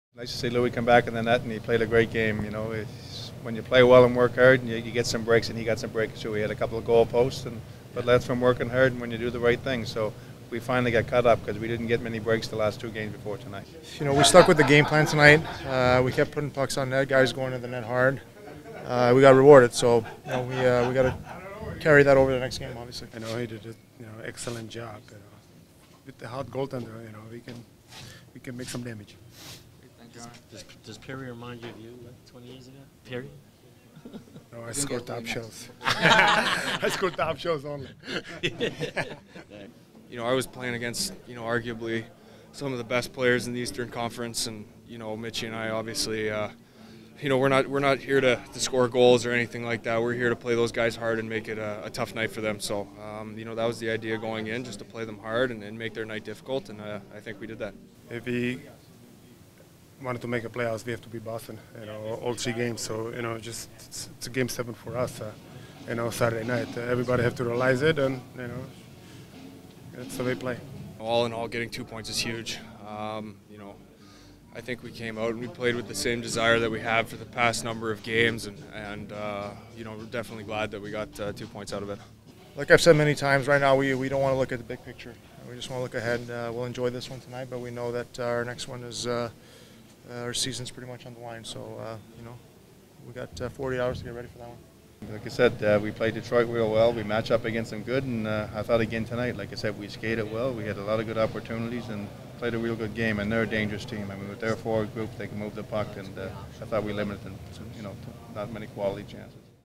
3/19/2015 Post Game Interviews
Interviews with Gerard Gallant, Roberto Luongo, Jaromir Jagr, and Erik Gudbranson